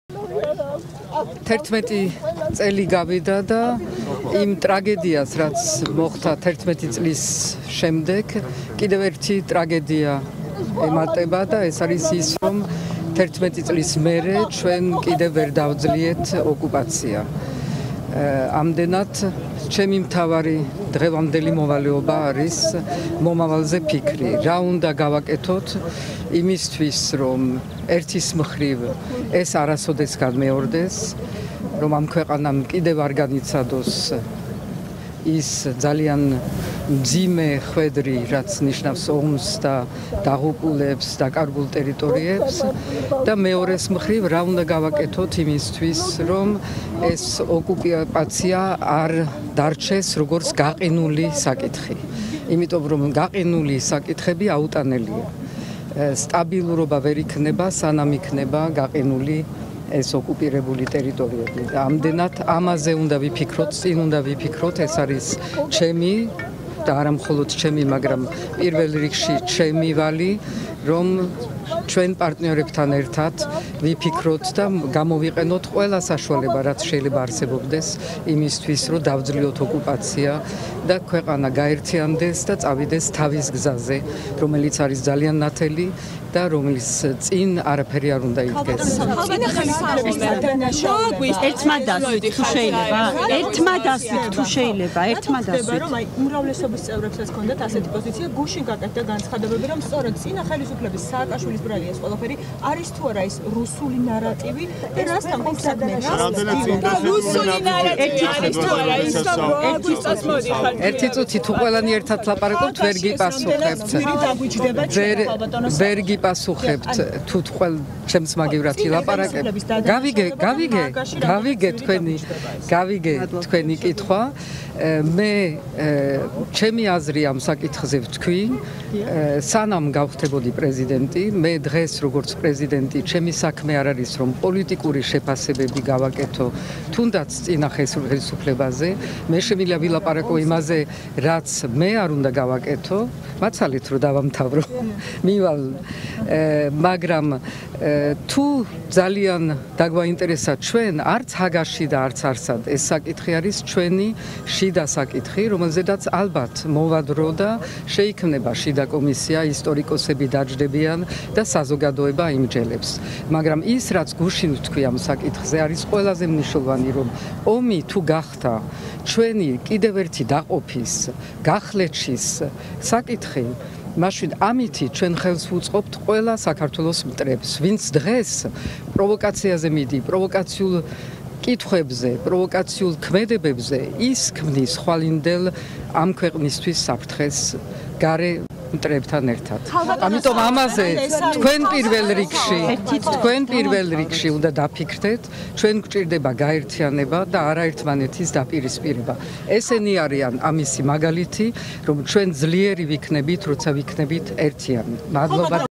„ყველაფერი უნდა გავაკეთოთ, რომ დავძლიოთ ოკუპაცია და ქვეყანა წავიდეს თავის დასავლურ გზაზე“
სალომე ზურაბიშვილი: „ყველაფერი უნდა გავაკეთოთ, რომ დავძლიოთ ოკუპაცია და ქვეყანა წავიდეს თავის დასავლურ გზაზე“